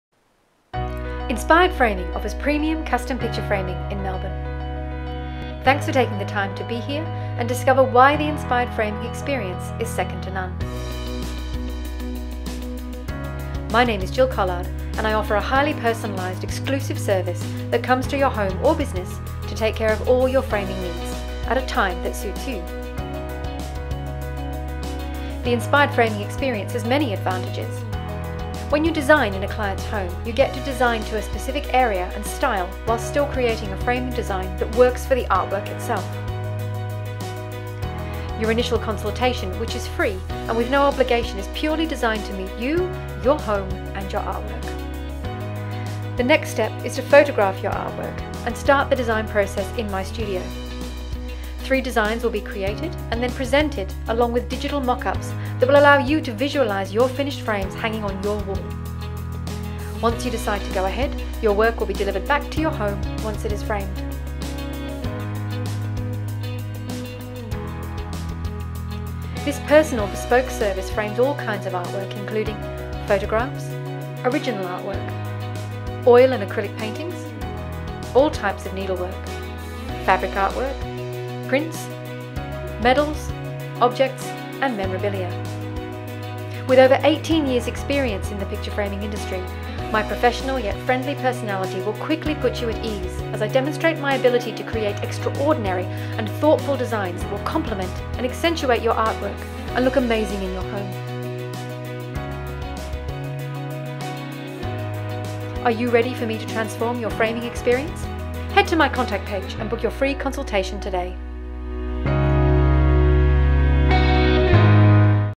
Experience Mobile Custom Picture Framing in Melbourne
Here's what to expect from the Inspired Framing experience.  Mobile custom picture framing in Melbourne.